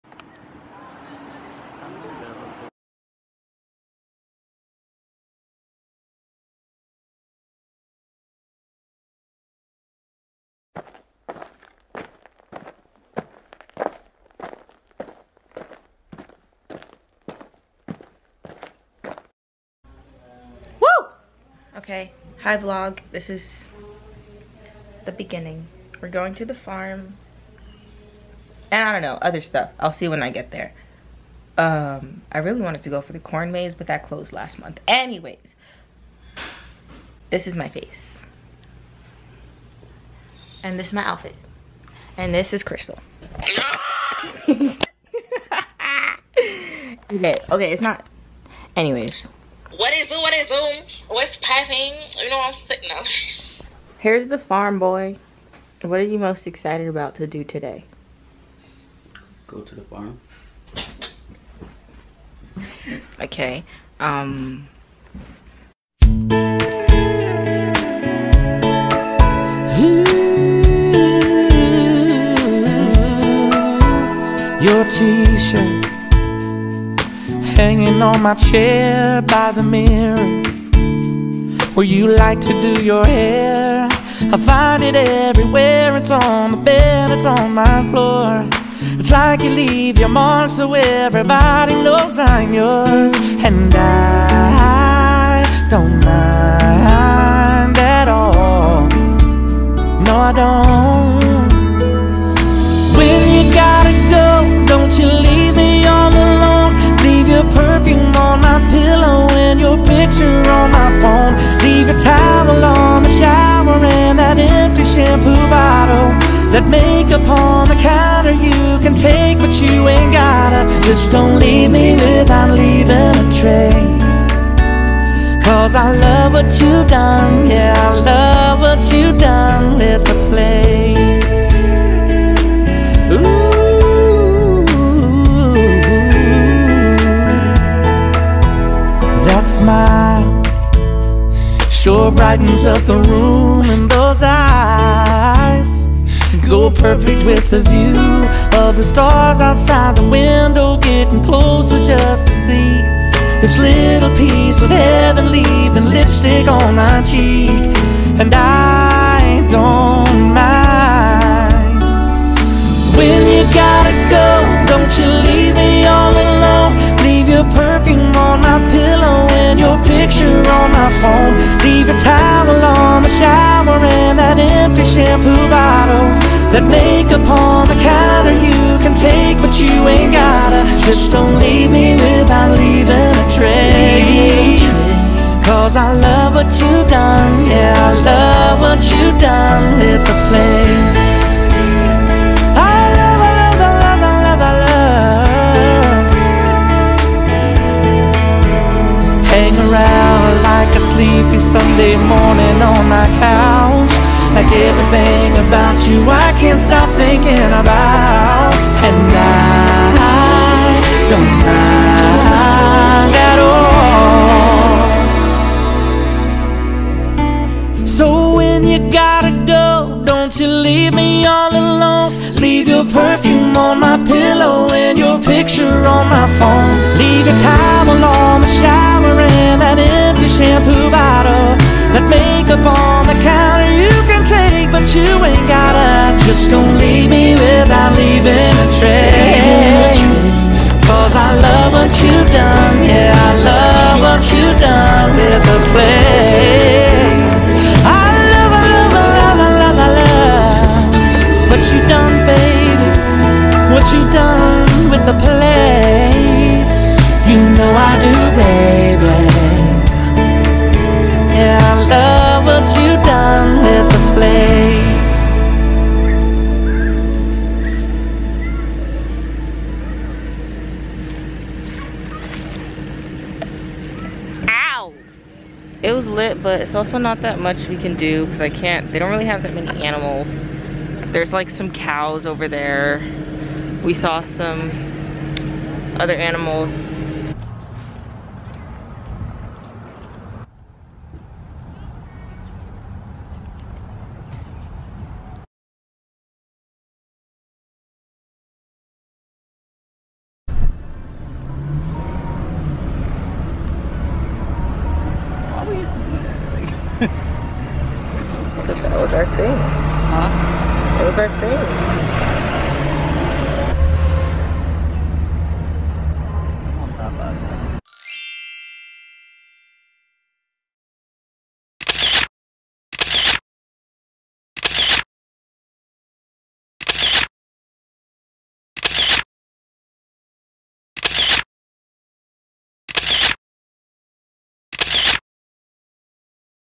New York's oldest farm | an unsolicited Rascal Flatt's music video
Nov 2020 archives for memorabilia. + Film photos.

please enjoy my unprecedented yet unsolicited music video to one of my favorite songs, this video makes me cry.